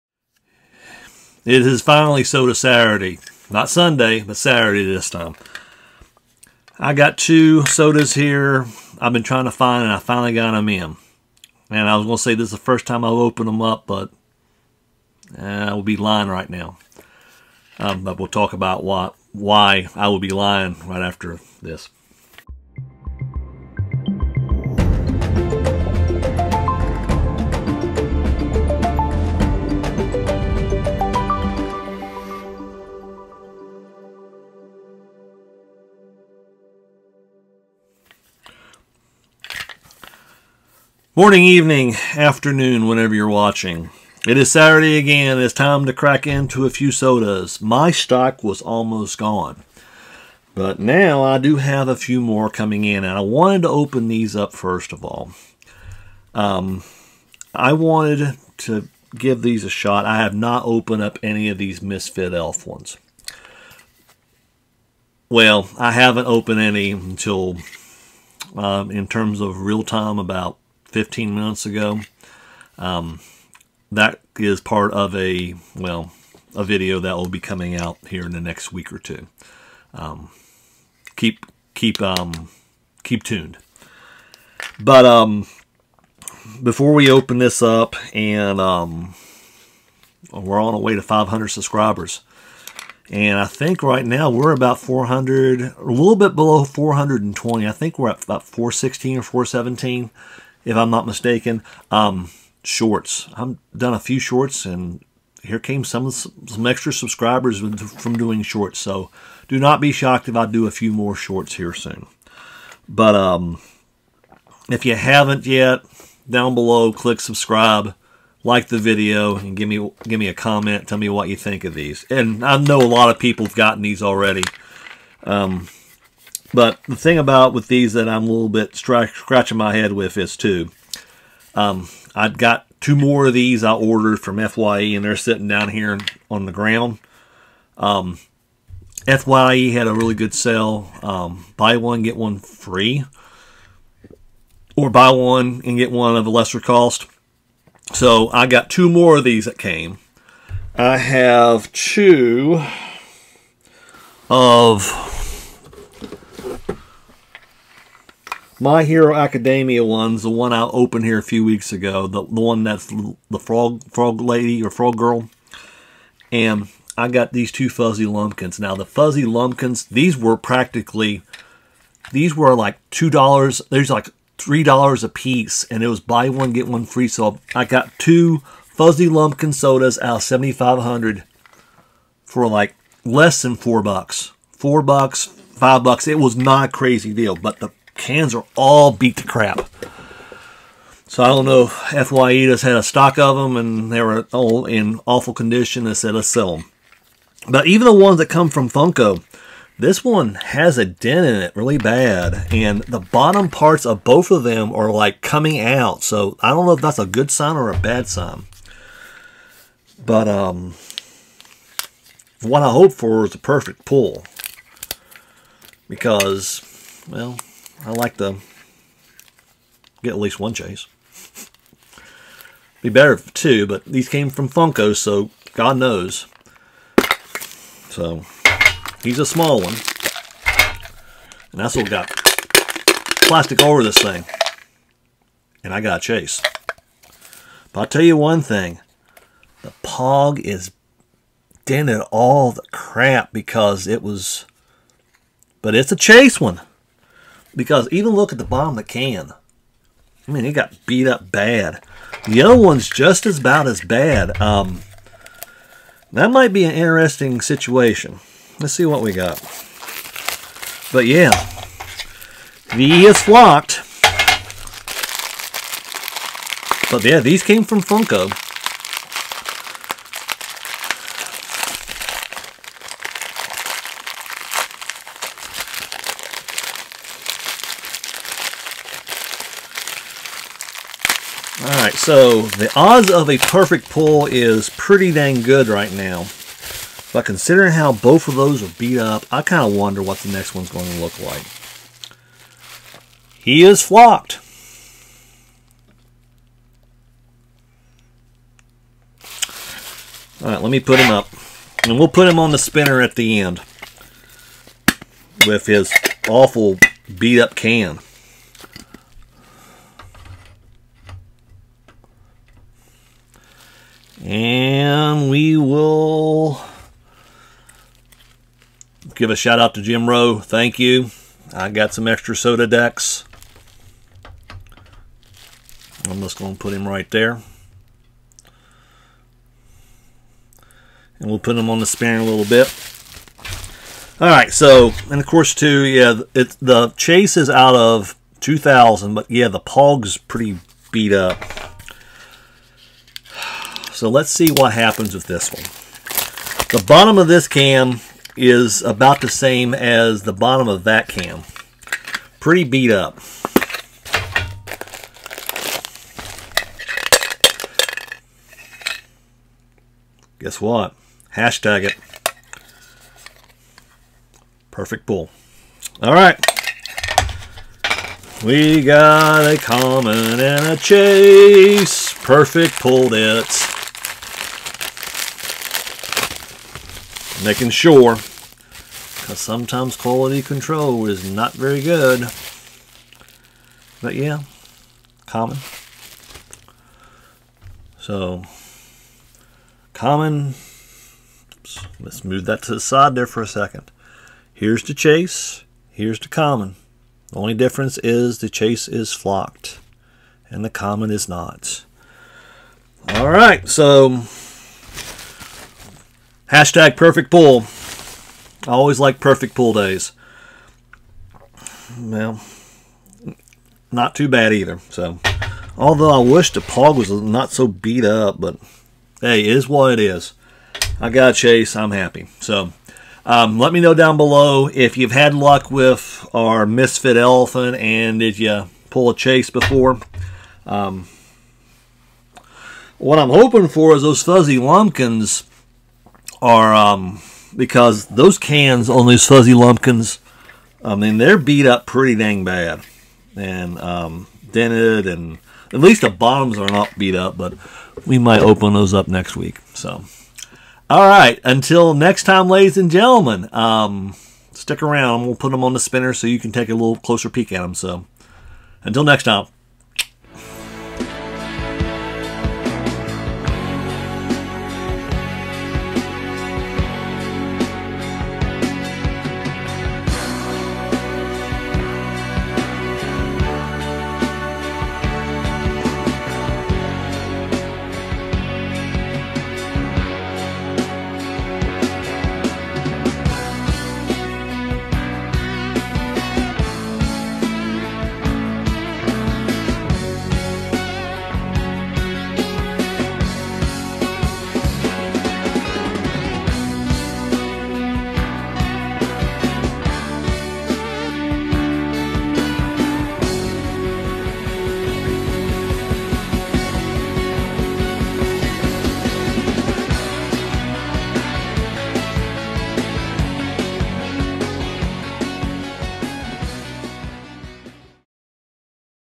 Soda Saturday - Misfit Elephant.... Finally!!!
Finally got a couple of Misfit Elephant sodas.  Let's see if I can pull a chase figure.